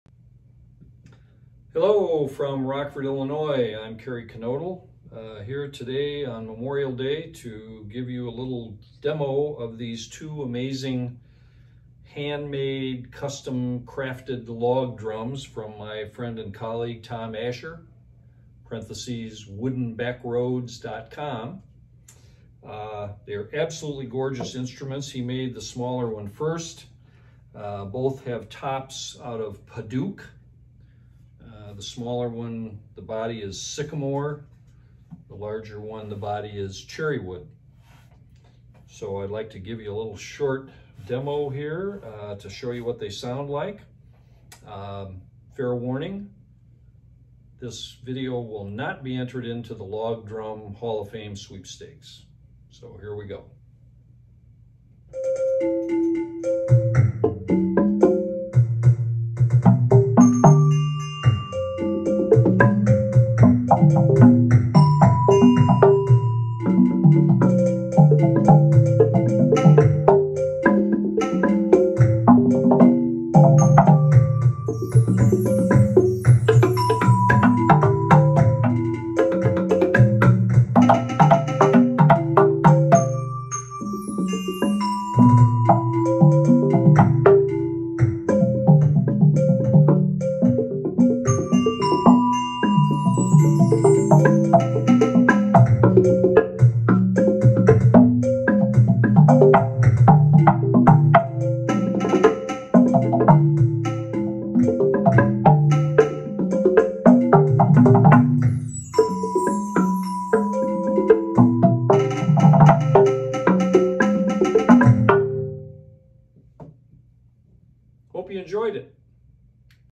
Log Drum demo
A short demo of two custom-made Log Drums from the shop of my friend Tom Asher Original drum track and improvised log drum.